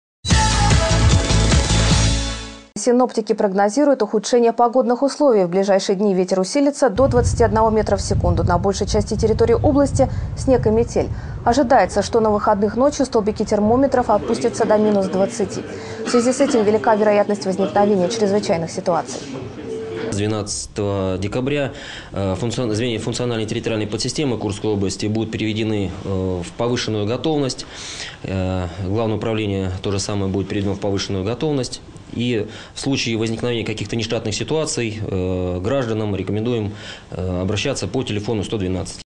Спасатели предупреждают курян об ухудшении погоды
Синоптики прогнозируют ухудшение погодных условий.